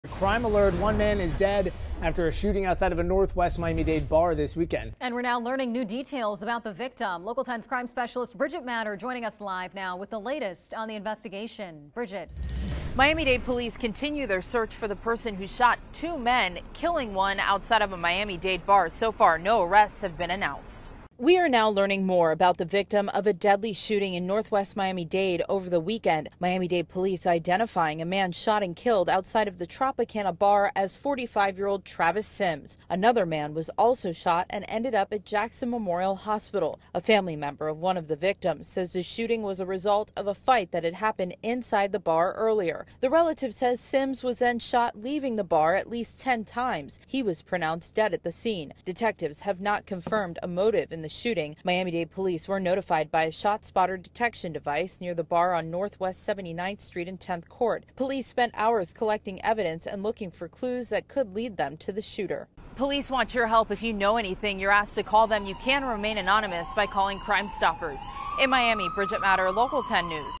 Bar fight in northwest Miami-Dade leaves 1 dead, police search for suspect
A man died and another was injured during a shooting on Sunday morning outside of the Tropicana Bar & Lounge in northwestern Miami-Dade County, police said.